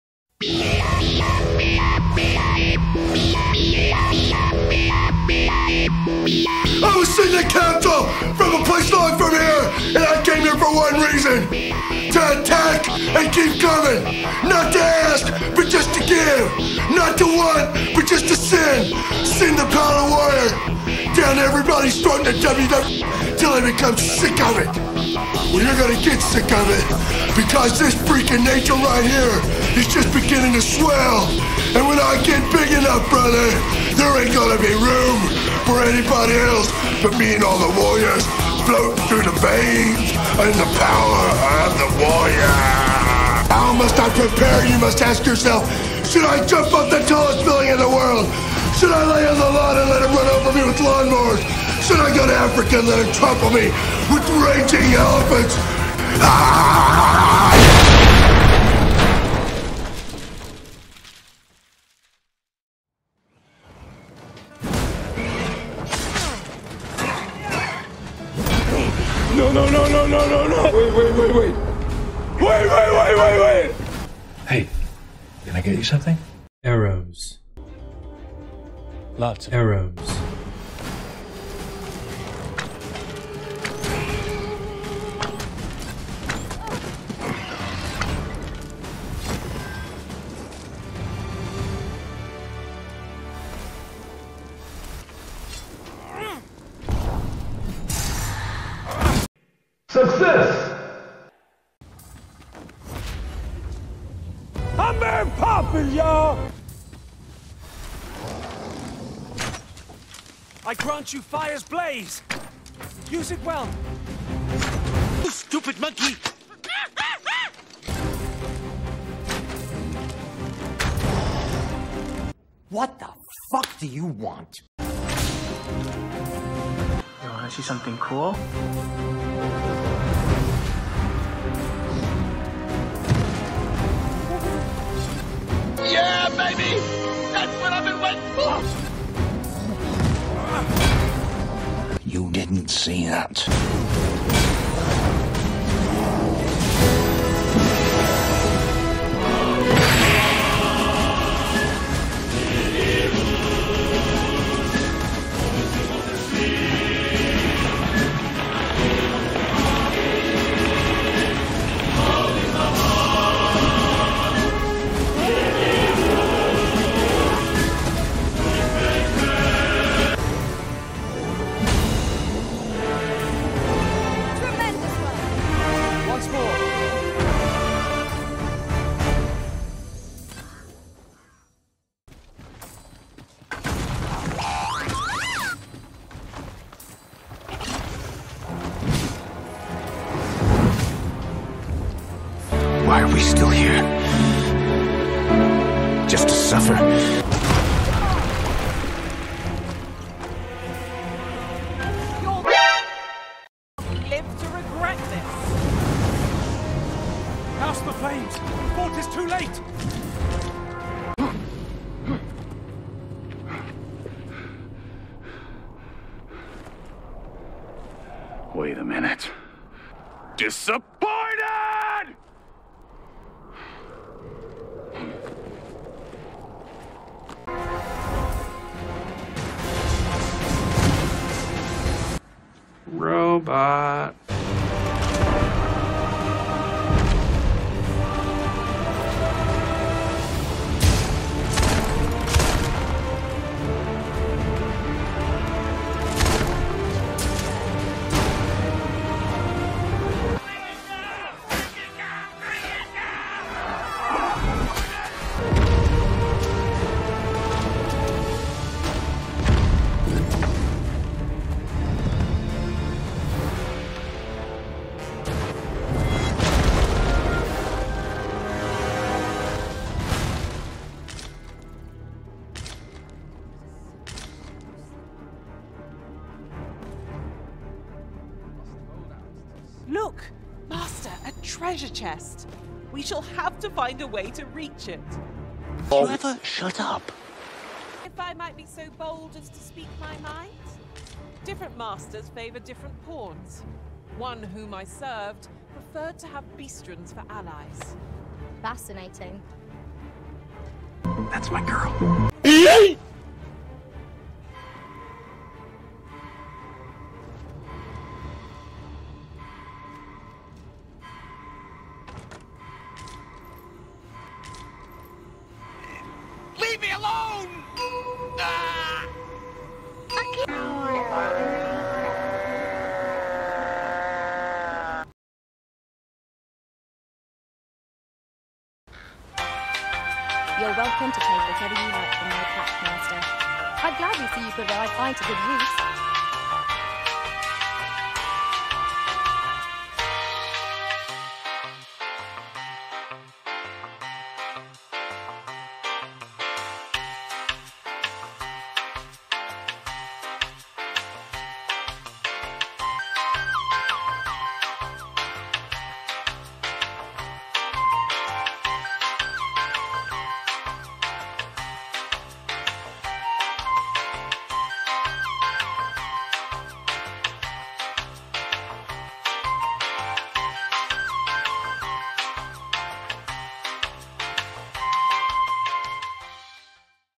Warfarer Warrior is OP (Gone Wrong)
This video showcases how I use the warfarer as a warrior main but also is not at all a serious video but I was inspired to do this. Also sort of encapsulates my feelings and experience while playing this wonderful game. Hopefully you like it :) I'm approaching the endgame (taking my time) so will be uploading some of those videos with commentary and such over the next week! Thanks for all of the support, everyone. Have a good one and give somebody a hug.